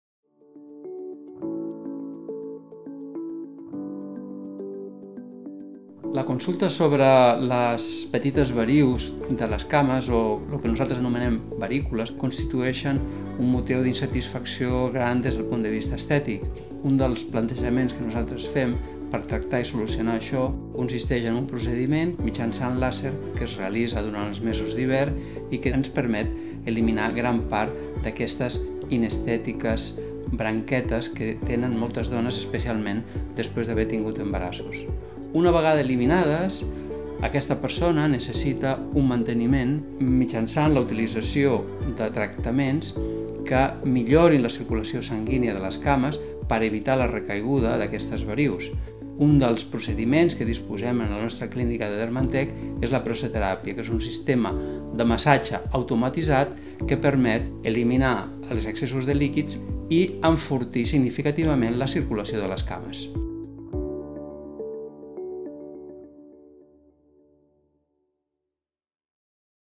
DermAndTek - Varius i Aranyes vasculars
El Dr. López Gil ens parla sobre com eliminar les varius i les aranyes vasculars amb tractament làser i la importància de realitzar un tractament de manteniment amb la pressoteràpia per aconseguir uns resultats excel·lents.